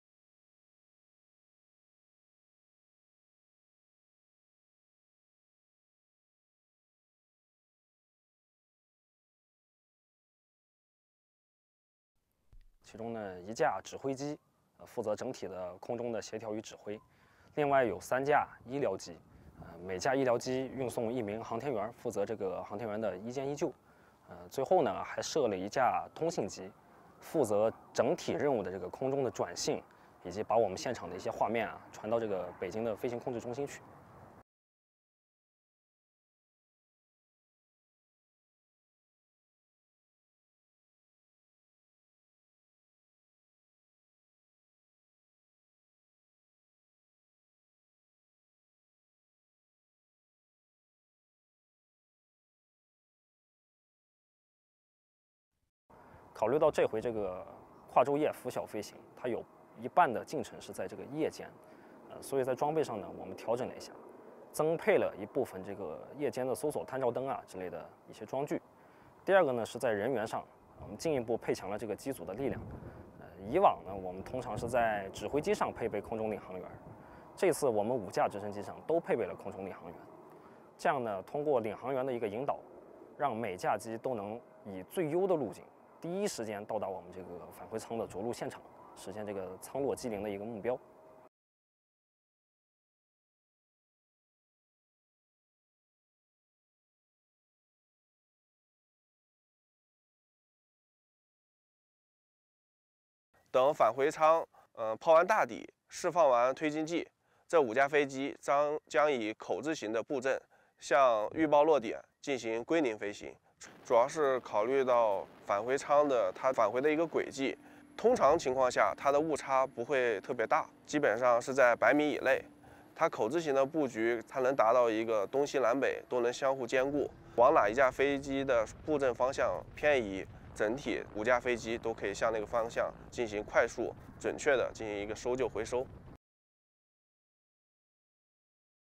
Aerial Rescue Team Steps up Preparations for Shenzhou-16's Return
China's aerial search and rescue team at the Dongfeng landing site in Inner Mongolia Autonomous Region have made full preparations for the return of Shenzhou-16 spaceship and the three astronauts aboard it from the Chinese space station.